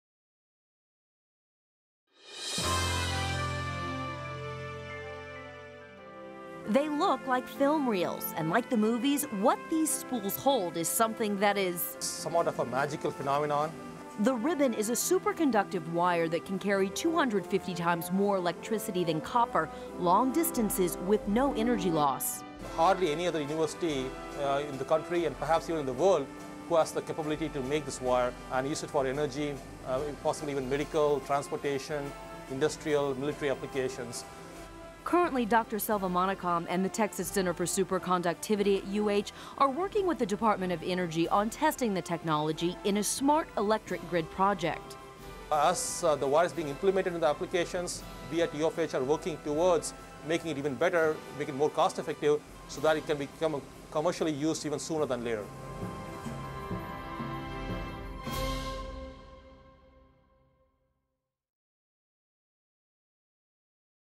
HoustonPBS UH Moment: Superconductive Wire
In 2010 the University of Houston received a five-year, $3.6 million grant from the state's Emerging Technology Fund to aid in the development and commercialization of products based on high-temperature superconductors. Superconductivity uses certain ceramic materials that are cooled with liquid nitrogen so they have no electrical resistance. Venkat "Selva" Selvamanickam, professor of mechanical engineering at UH, talks about the work that UH and SuperPower, Inc., are doing to further superconductor products, including a wire that is being tested by the U.S. Department of Energy in smart electric grid project.